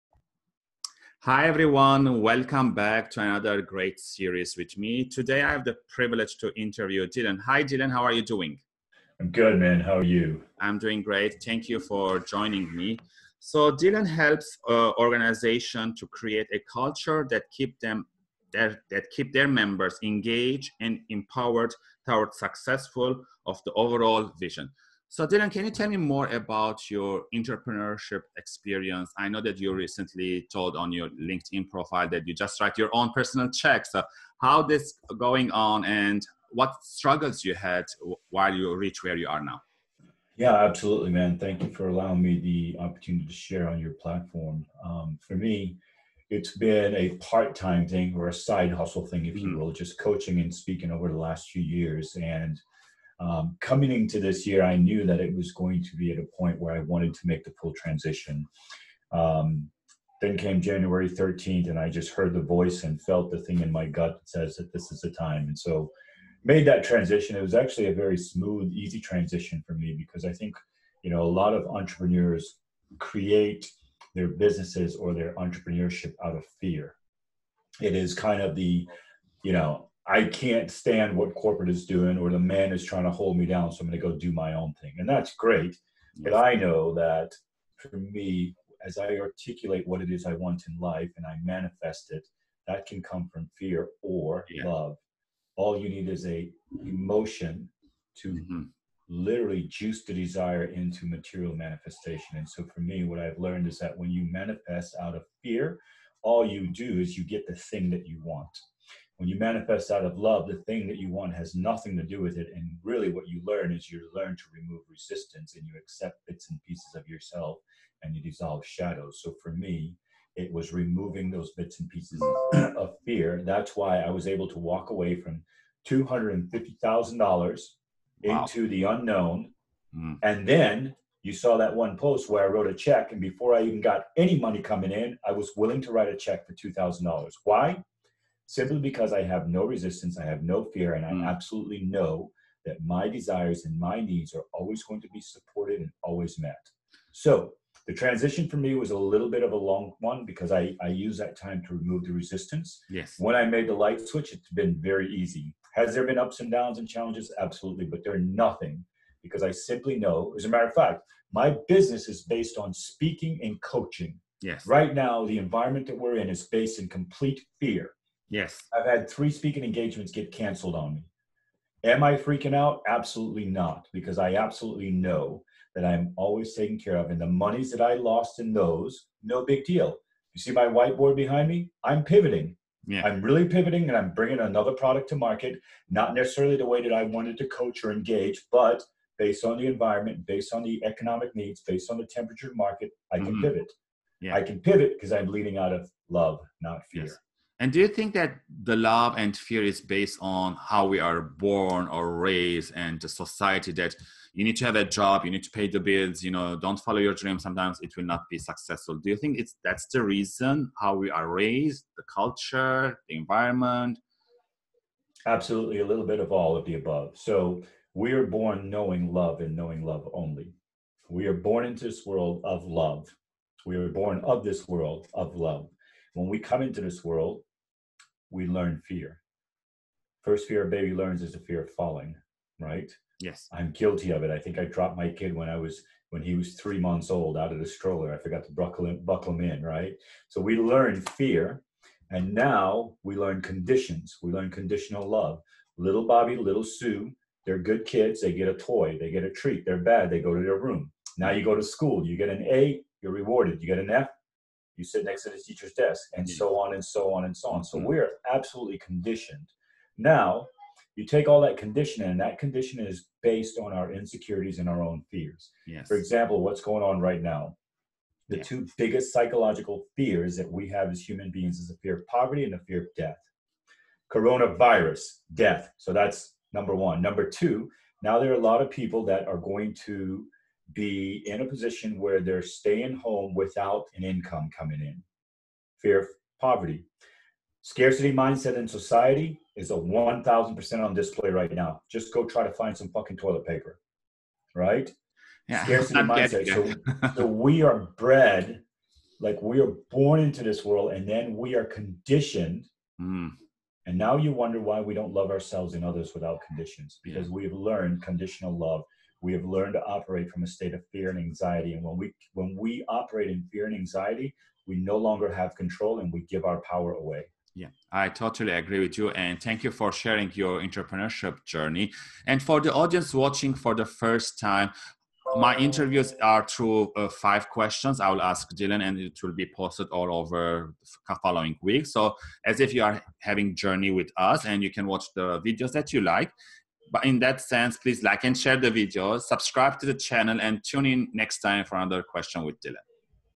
Interview series with Dylan Ali
My coaching and speaking were a part-time thing or a side hustle thing, over the last few years. Coming into this year, I knew that at some point, I wanted to make the full transition. On Jan 13, my gut was saying that this is the time and I made that transition. It was actually a very smooth, easy transition for me because I knew a lot of entrepreneurs create their businesses or their entrepreneurship out of fear.
For me, as I articulate what it is, I want in life and I manifested, that can come from fear or love. All you need is a motion to literally juice the desire into material manifestation. For me, what I've learned is that, when you manifest out of fear, all you do is you get the thing that you want. When you manifest out of love, the thing that you want has nothing to do with it. What you learn is, to remove resistance and you accept bits and pieces of yourself and you dissolve shadows.
For me, it was removing those bits and pieces of fear, that's why I was able to walk away from $250,000 into the unknown. Then you saw that one post where, I wrote a check, and before I even got any money coming in, I was willing to write a check for $2,000. Why? Simply because I have no resistance, I have no fear. I absolutely know that my desires and my needs are always going to be supported and always met. The transition for me was a little bit of a long one because I use that time to remove the resistance.
Have there been ups and downs and challenges? Absolutely. My business is based on speaking and coaching, right now the environment that we're in is based in complete fear. I've had three speaking engagements get canceled on me. Am I freaking out? Absolutely not, because I absolutely know that, I'm always taking care of, and the monies that I lost is not a big deal. You see my whiteboard behind me, I'm really pivoting and I'm bringing another product to market, not necessarily the way that I wanted to coach or engage, but based on the environment, based on the economic needs, based on the temperature market. I can pivot because I'm leading out of love, not fear.
We are born knowing love and knowing love only. We are born into this world of love. When we come into this world, we learn to fear.
We're conditioned, you take all that condition is based on our insecurities and our own fears. For example, what's going on right now, the two biggest psychological fears that we have as human beings these days are; fear of poverty and fear of death. Coronavirus death, that's number one.
Number two, now there are a lot of people that are going to be in a position where they're staying home without an income coming in, fear of poverty.